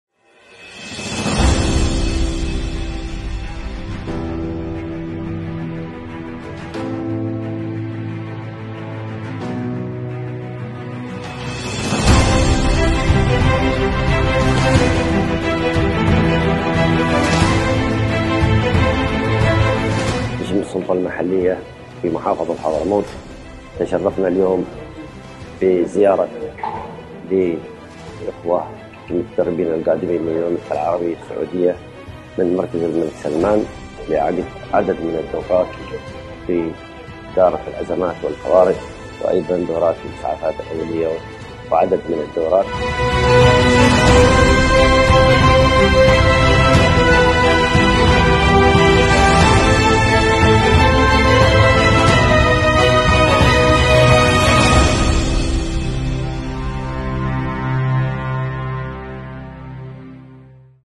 دورات السلامة والإنقاذ في المكلا
مشاركتي في تدريب الصحة والسلامة والإنقاذ مع مركز الملك سلمان للاعمال الانسانية بالمكلا باليمن الشقيق